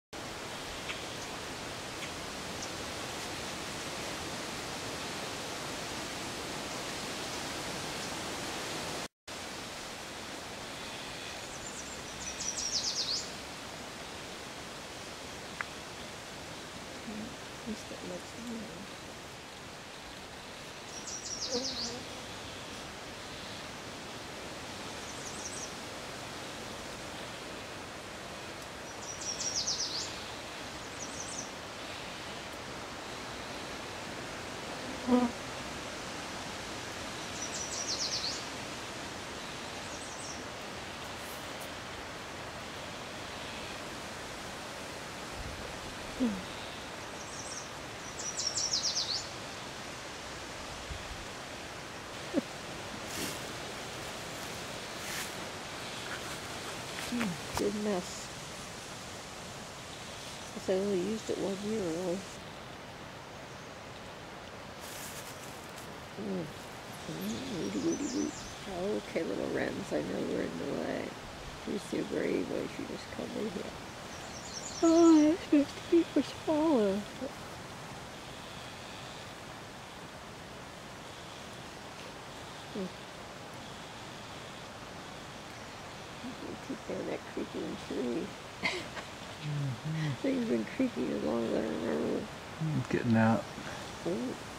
thbv2june11.wmv
beaver in a small getting willow leaves to eat